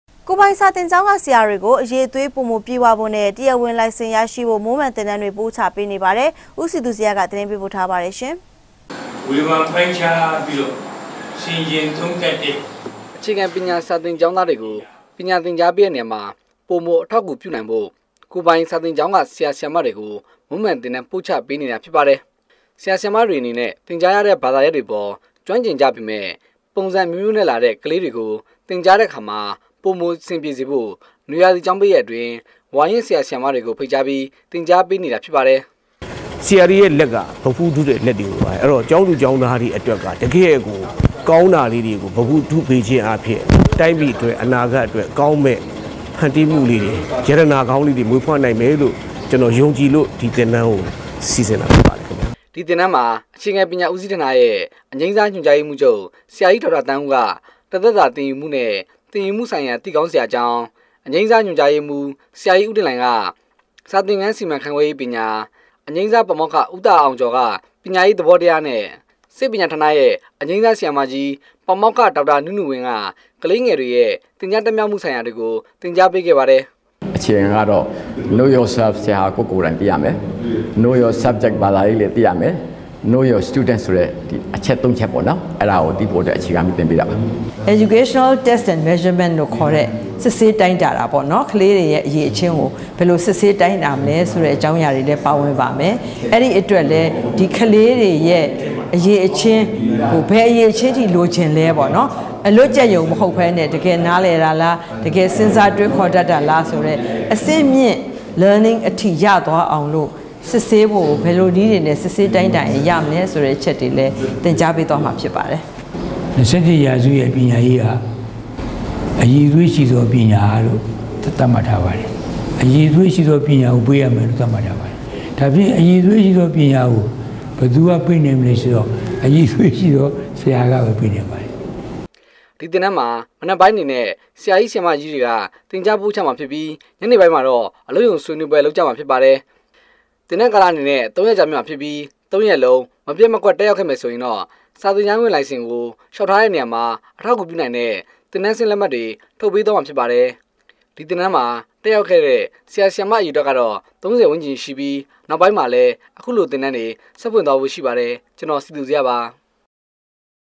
DVB - ကိုယ္ပိုင္စာသင္ေက်ာင္းမွ ဆရာေတြကုိ မြန္းမံသင္တန္းပုိ႔ခ်
DVB TV -
ကိုယ္ပိုင္စာသင္ေက်ာင္းက ဆရာေတြကို အရည္အေသြးပိုမိုျပည့္ဝဖို႕နဲ႕ တရားဝင္လိုင္စင္ရရွိဖို႕ မြမ္းမံသင္တန္းေတြ ပို႔ခ်ေပးေနပါတယ္။ ဦးစည္သူေဇယ် သတင္းေပးပို႔ထားပါတယ္။ ရိုက္ကူး- စည္သူေဇယ်